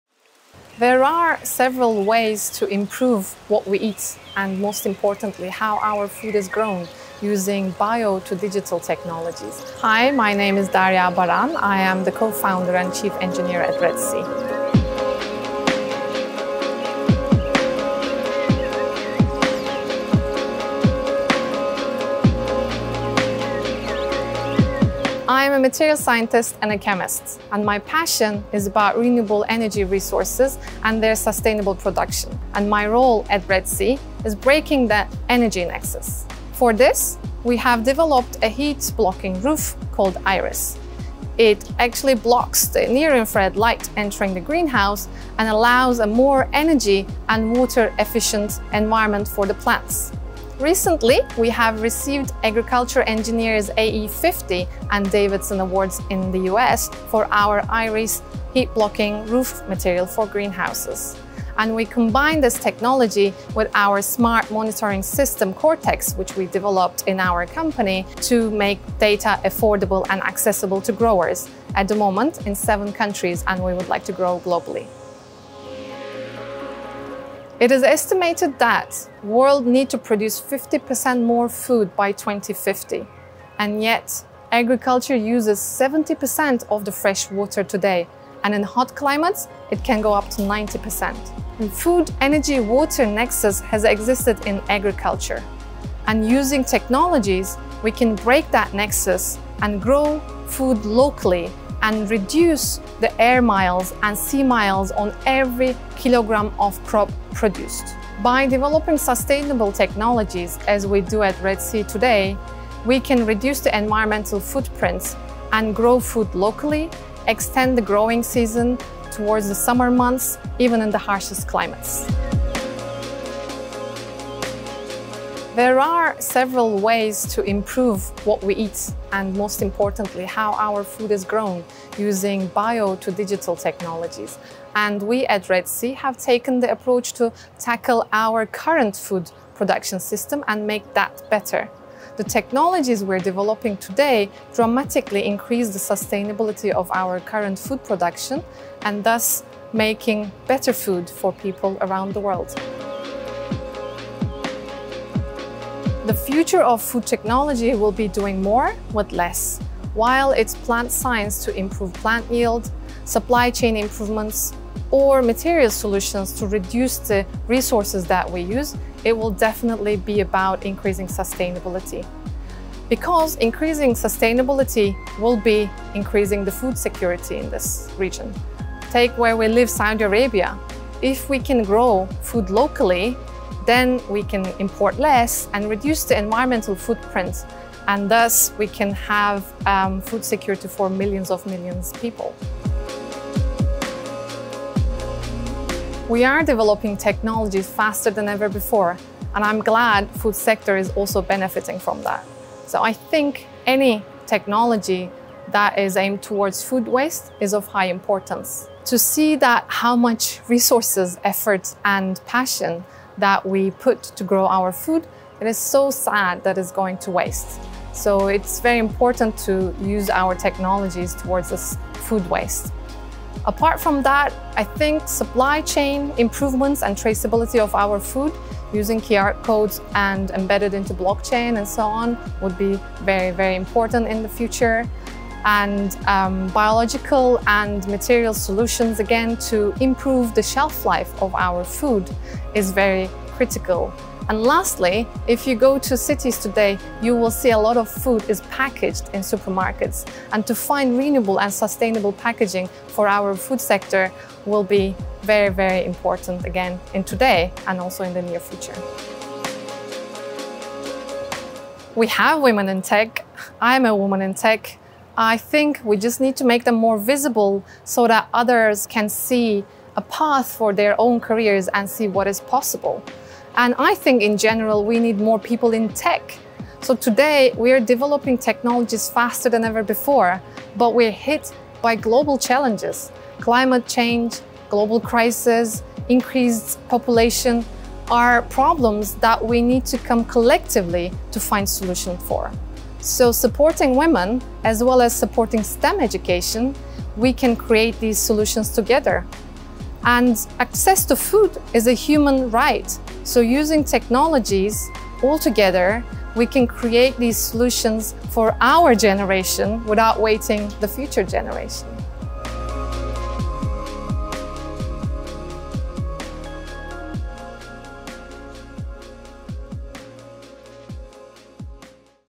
Is technology the answer for food security in the Middle East?
Talking about opportunities in the food tech sector and its impact on food security, RedSea co-founder and Chief Engineer Derya Baran explains how biotech and digital technologies can improve what we eat and, most importantly, how it's grown. To become less reliant on freshwater use in water-scarce regions and harsh climates, Baran's team has developed a heat-blocking roof, allowing more energy and water-efficient environment for plants, combined with their AI-enabled remote monitoring technology.